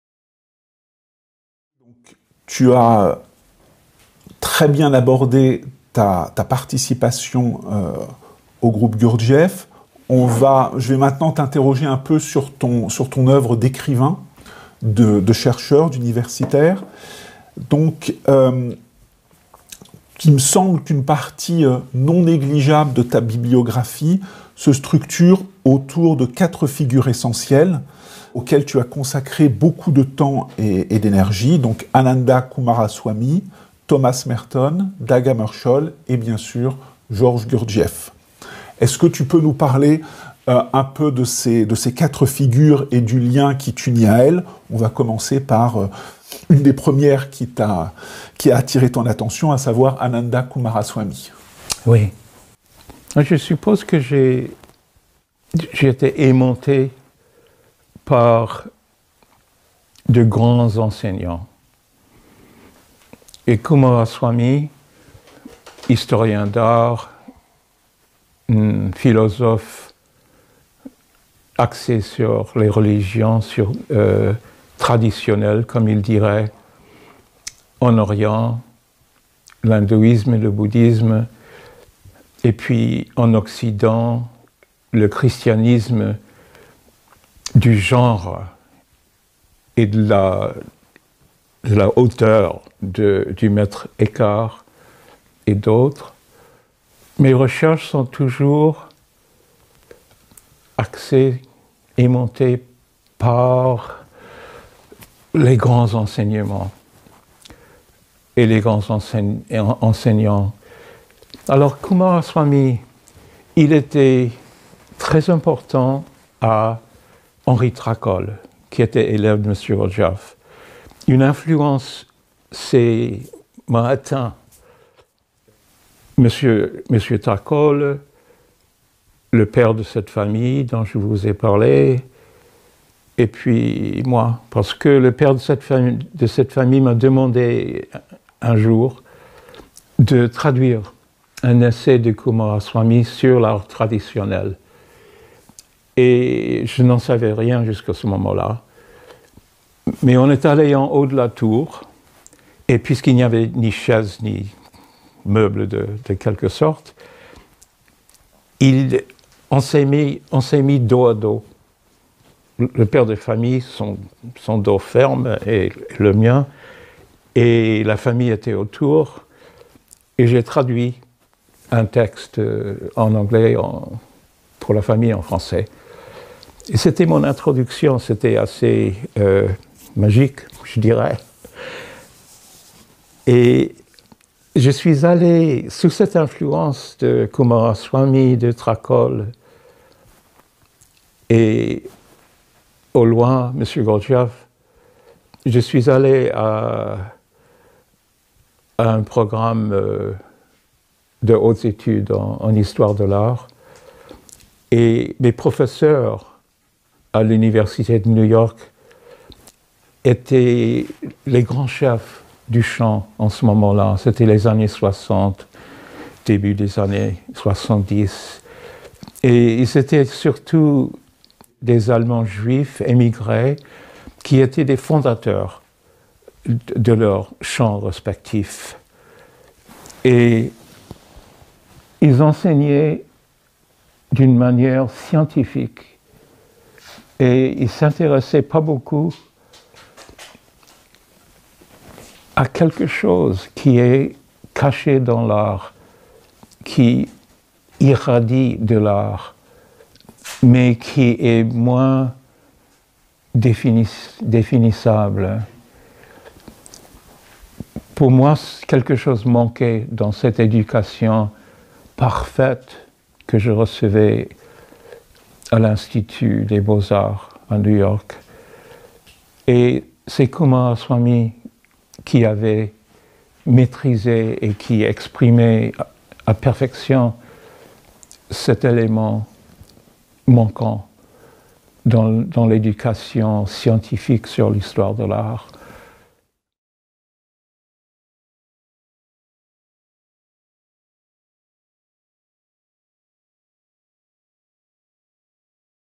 Pour Coomaraswamy : l’art est irradiant | BAGLIS TV
"L’art est irradiant et ses historiens ignorent cette dimension transformatrice...". Un des apports de l'oeuvre de Coomaraswamy nous précise Roger Lipsey.
Extrait d'un exposé intitulé « De Coomaraswamy à Gurdjieff : étapes d’une éducation esthétique et spirituelle » par Roger Lipsey et Frédéric Blanc.